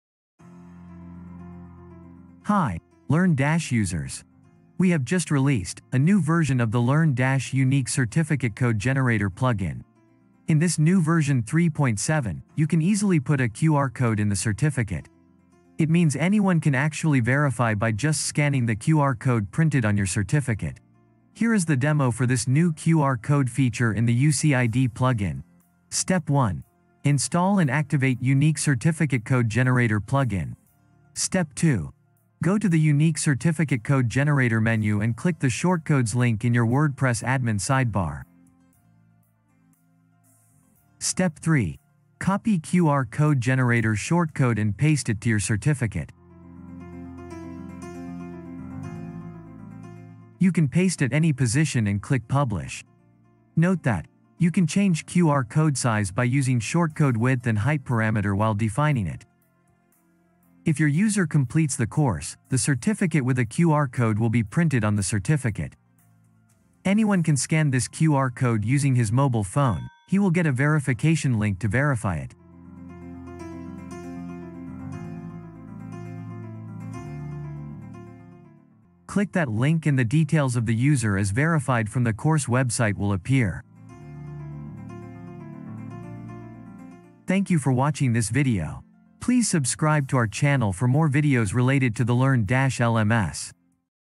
Verify LearnDash Certificate with QR Code in Printed on It
In our newest version 3.7.5 of the Learndash Certificate Unique Identification Code plugin, now you can create a course or quiz certificate with QR code printed on it. Anyone, who wants to verify if the certificate is authentic, should just scan the QR code and the verification result will appear on mobile.

Features of Learndash Certificate Unique ID Plugin
1.Create a unique id every time a certificate is generated for a user. This new unique ID will be associated with the user who generated the certificate.
2. The Admin of the site will get a list of the certificates generated with a user name, course name, user, and date of generation.
3. A shortcode for creating a mobile responsive verification page where anyone can verify the certificate.
5. The search page for issued certificate now in any language that Learndash supports [watch the video below]
6. Now the hyperlink for user id is created and can be printed on the certificate or shared on Linkedin profile or by mail.
7. The verification page will incorporate the photo of the user, provided he has created a profile picture.
8. The admin can also change fonts used on the verification page to a certain extent.
Now, a person can also print the certificate directly from the verification page.
9. The new version gives the admin power to show or not to show certain details on the verification page. See the image below